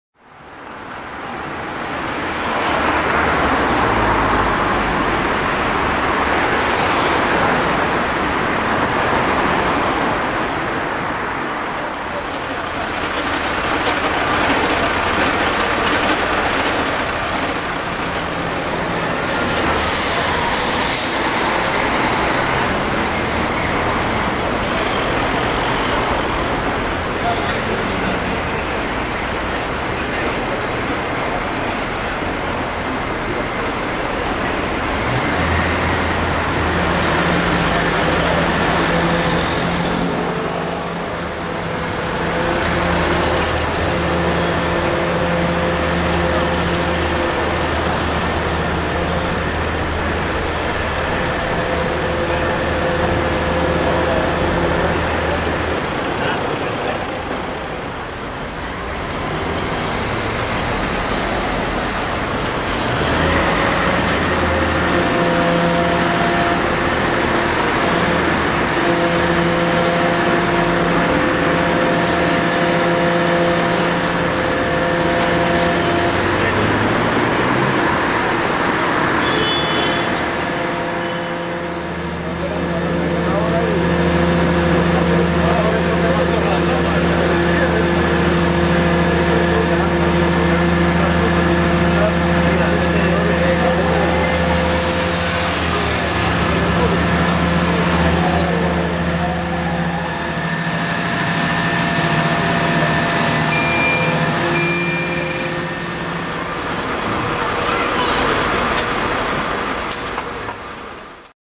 Conato incendio en Vigo 23-11-2013
Sobre las 14:00 horas, los bomberos acuden al número 24 de Travesía de Vigo, donde inspeccionan por dentro y por fuera el edificio, abandonando el lugar poco tiempo después, al no ser necesaria intervenciones mayores.